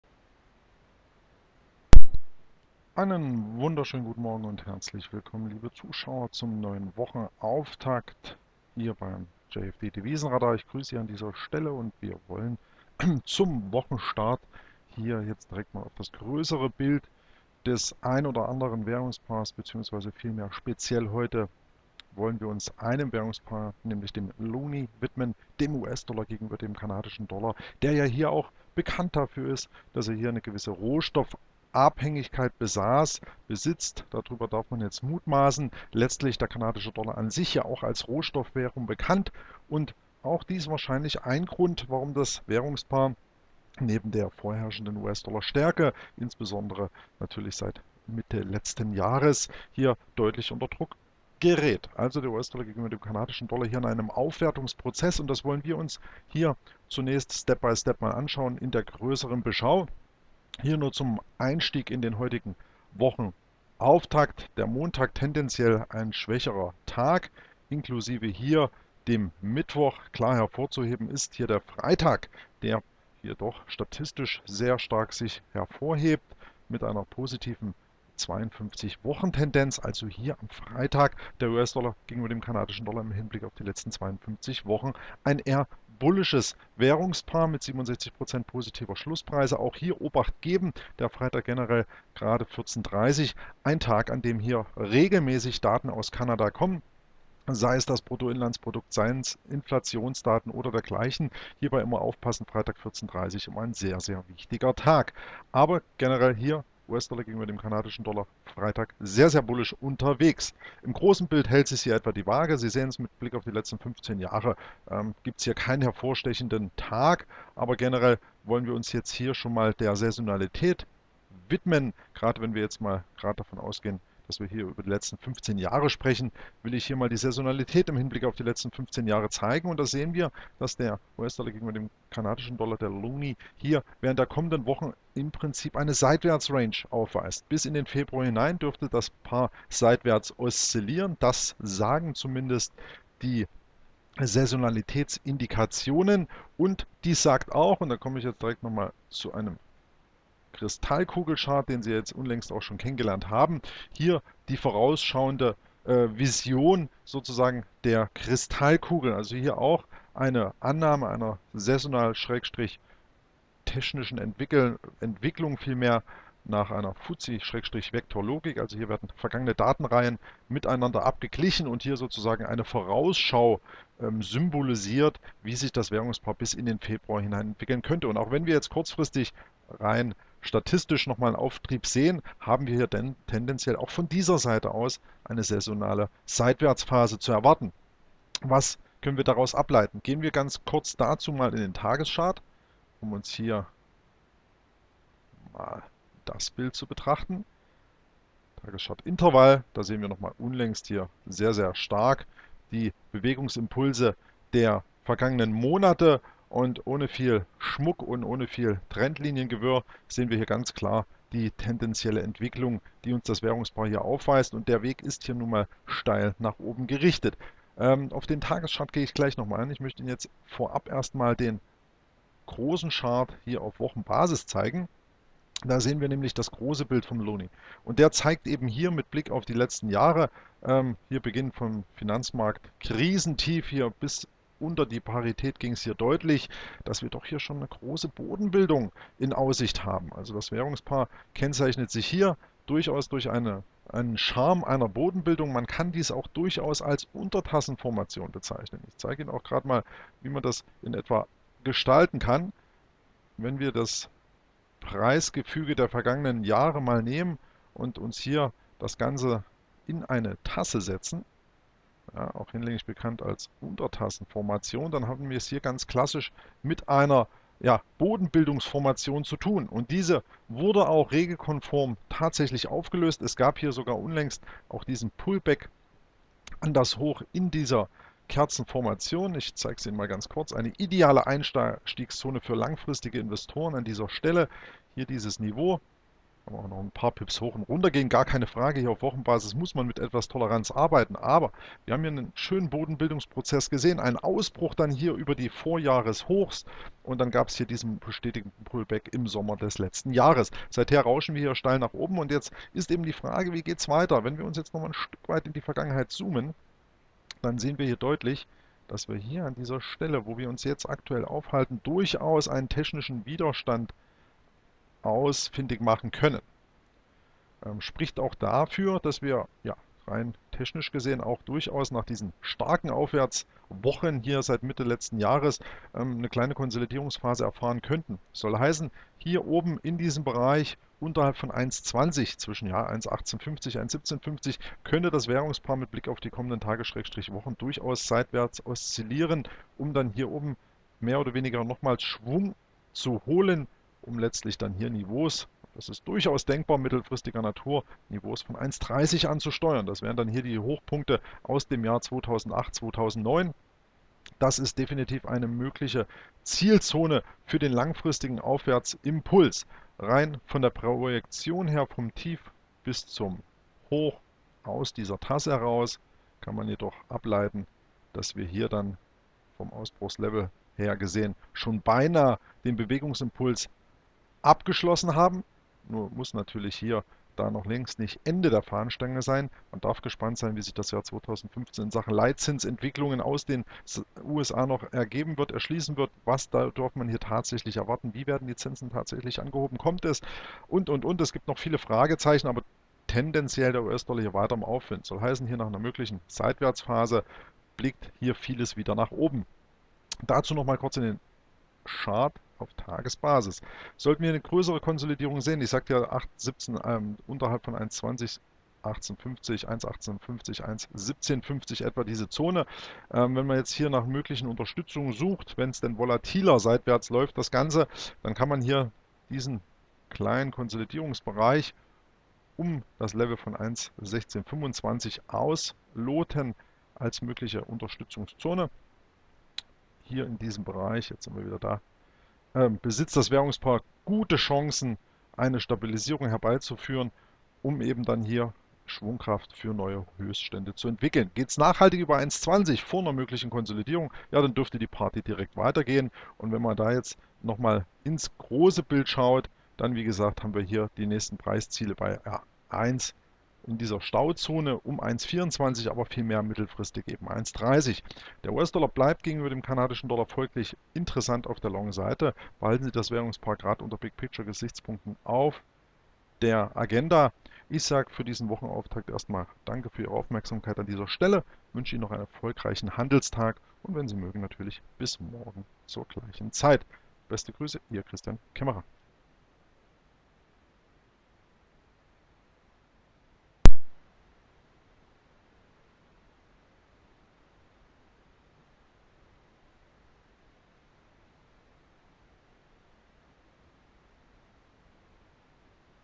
JFD Devisenradar: USD/CAD (Loonie) Big Picture
Der USD/CAD im großen Bild sowie mit der Beschau für die kommenden Wochen. Was darf man erwarten?